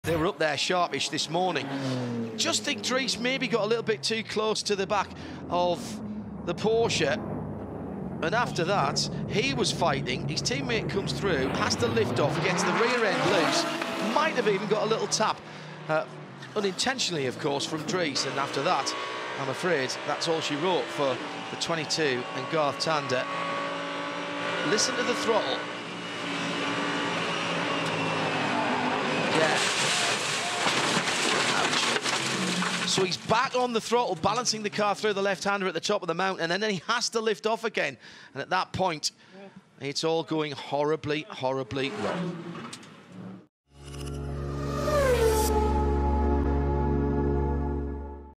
Bathurst 12H: Garth Tander crash
Supercars ace Garth Tander gets loose and takes his Audi R8 for a hard impact during the Bathurst 12H.

Not available in Australia/NZ